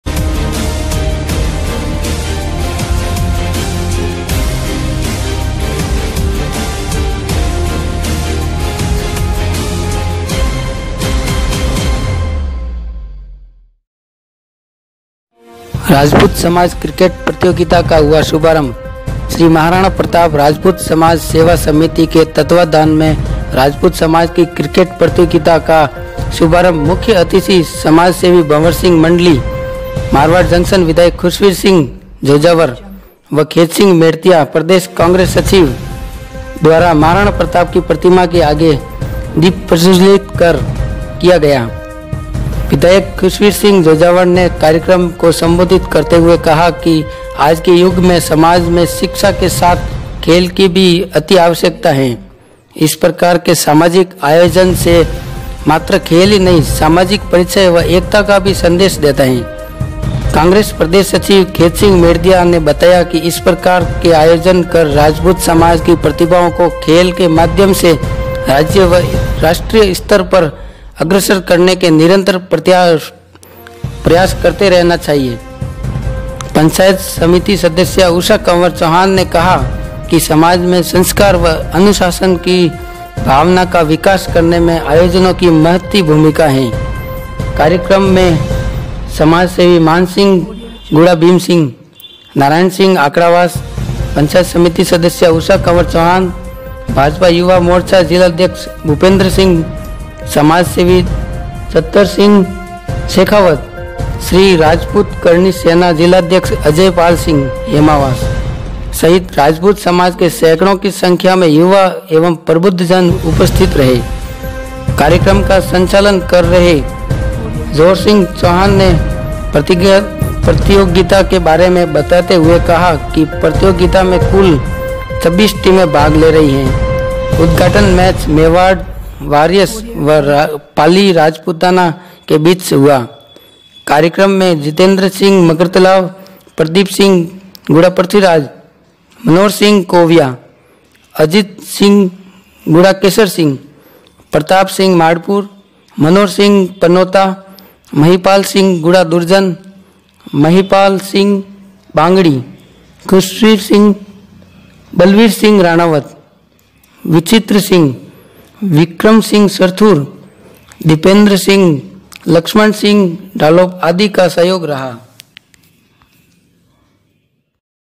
रानी-नाडोल में राजपूत समाज की ओर से आयोजित महाराणा प्रताप क्रिकेट प्रतियोगिता का हुआ शुभारंभ।
JKNEWS SBSE TEZ राजस्थान